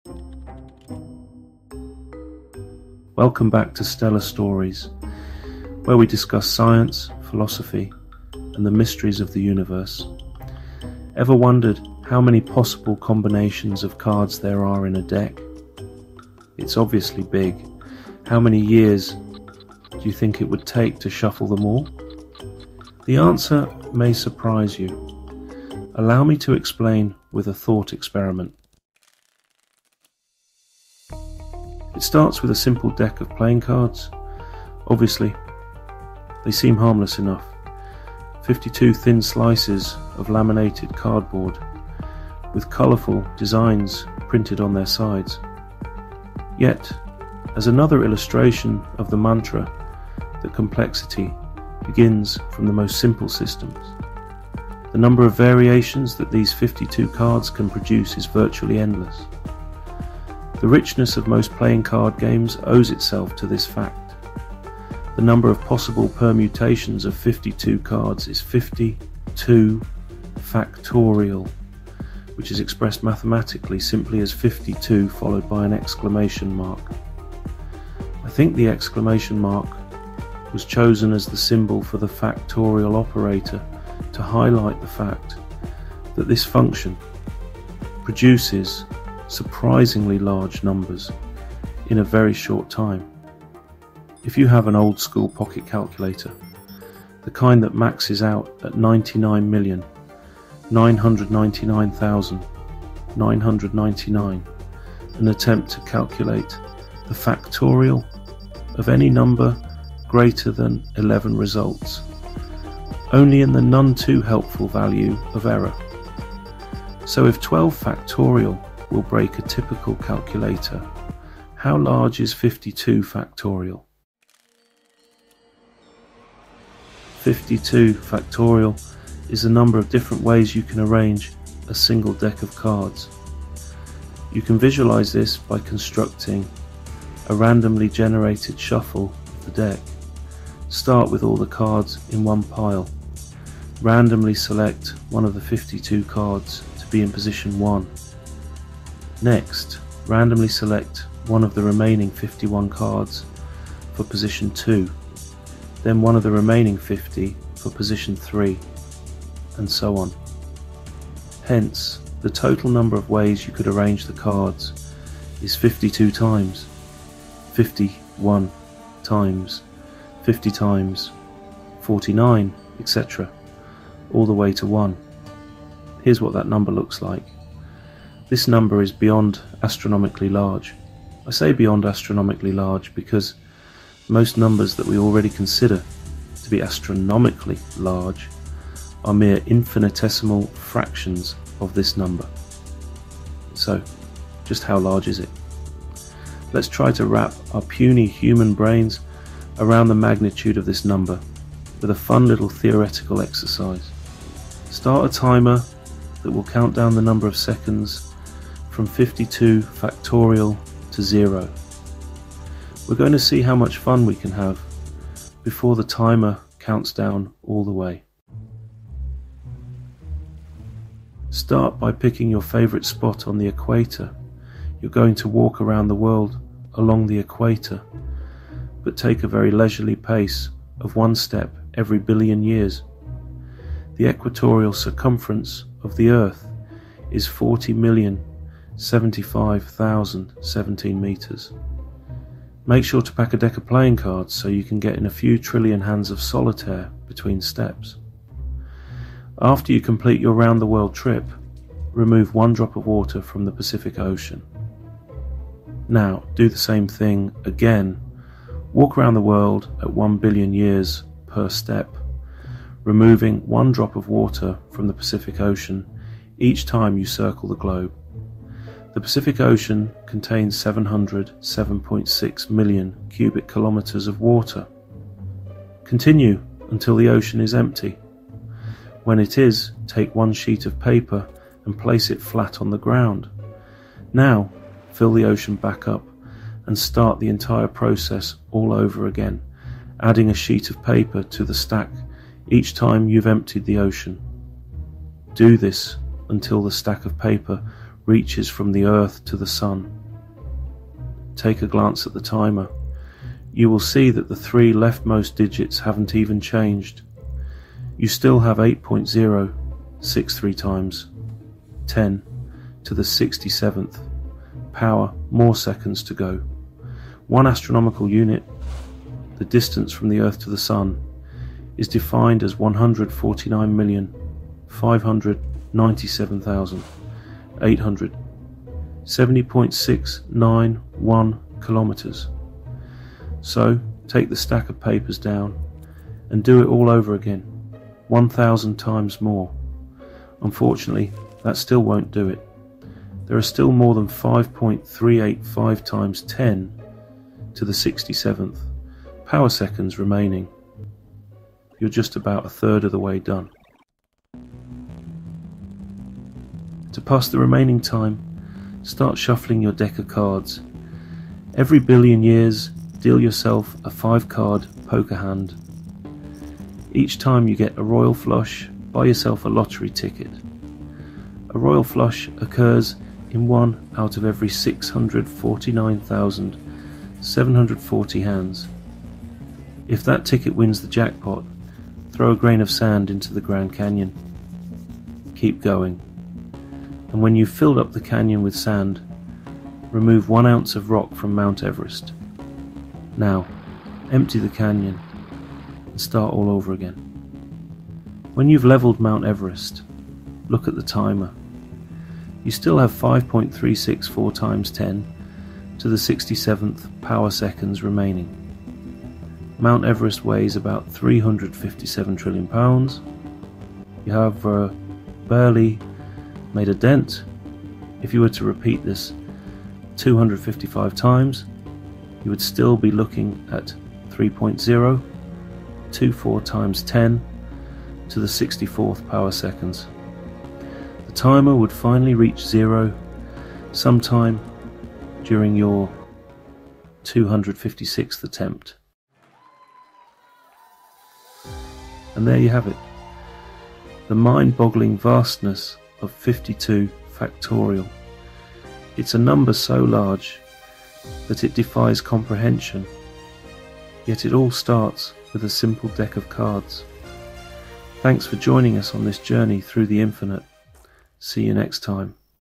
The amazing scale of 52! How long would it take to deal all combinations of cards?
This is the amazing story of 52 factorial, the number of different combinations of cards in a deck. This is my take on a reddit post I read about a decade ago. I tried to find it to  give the OP the credit they deserve but could only find reposts of it. Enjoy!